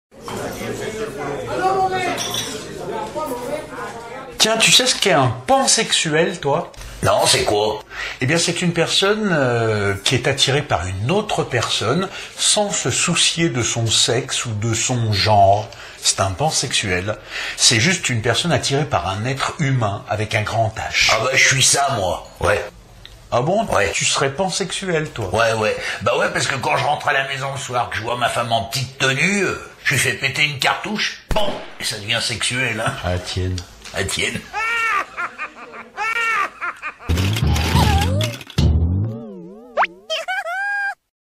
Brèves de comptoir - Etre "pansexuel"
Aujourd'hui dans notre bistro lyonnais on parle du "pansexuel". C'est quoi ce nouveau mot au juste ?